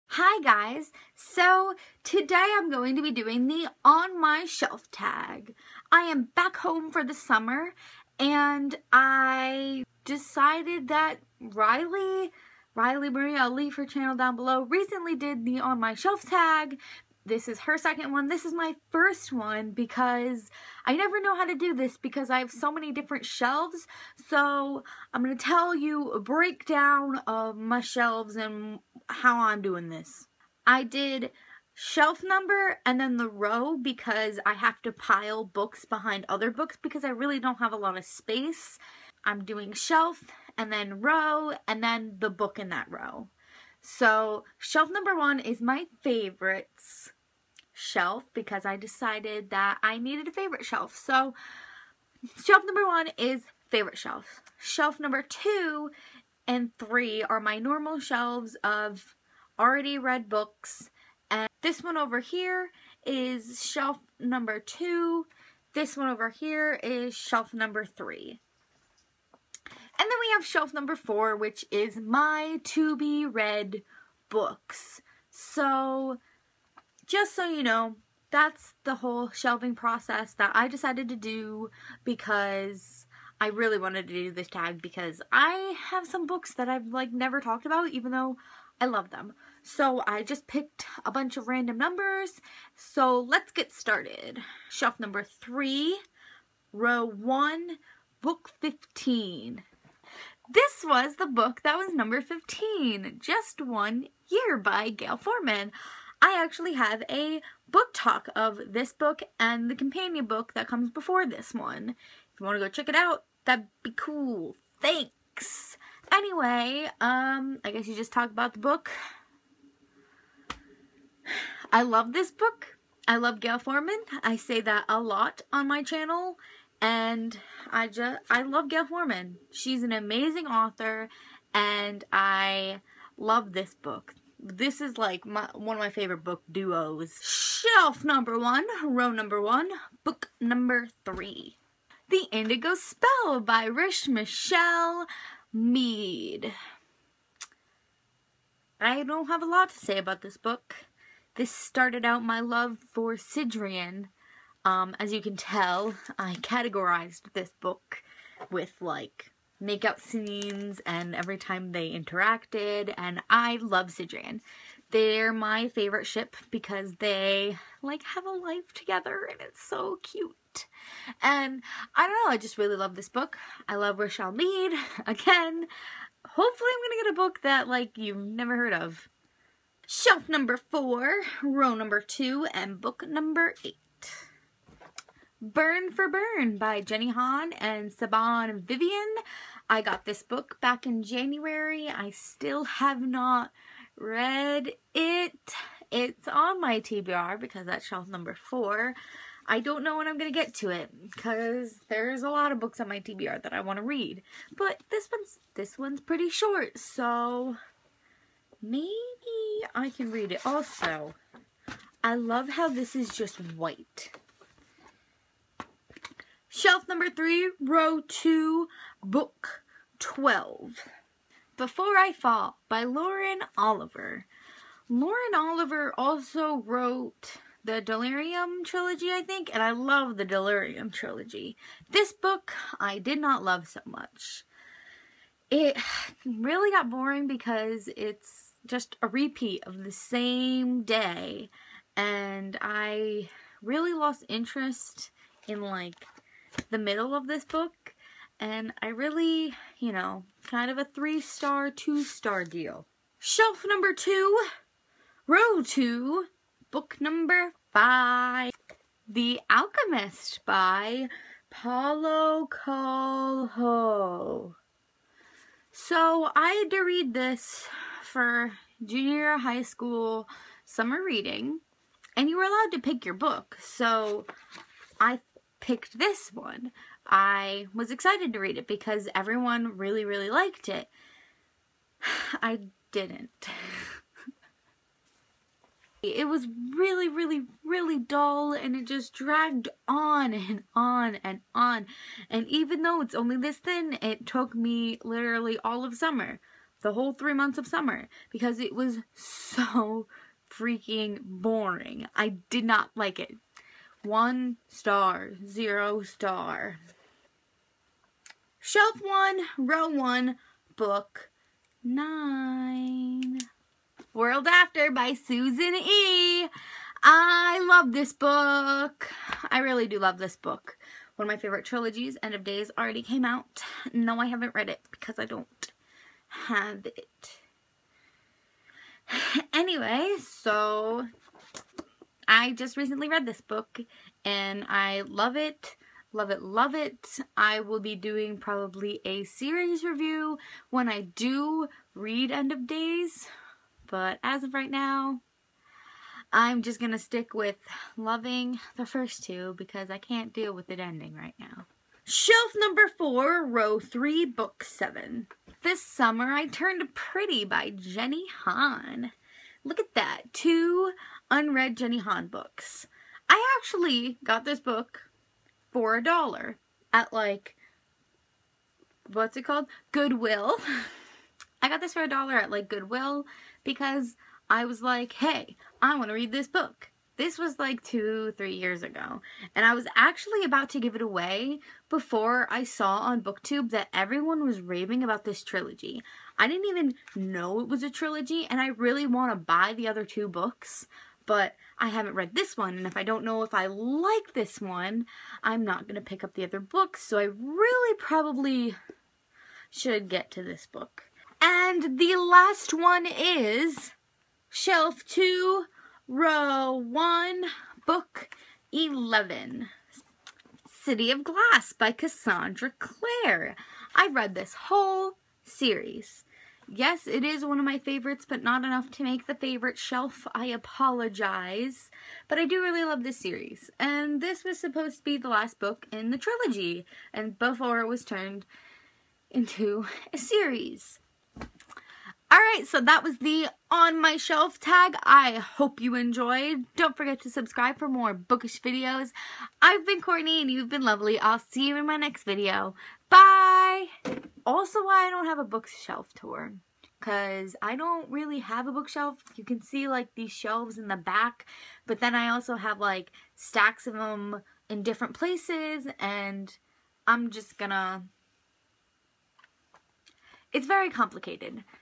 On my Shelf Tag
Where I really don't know anything about the books on my shelf and I'm confused.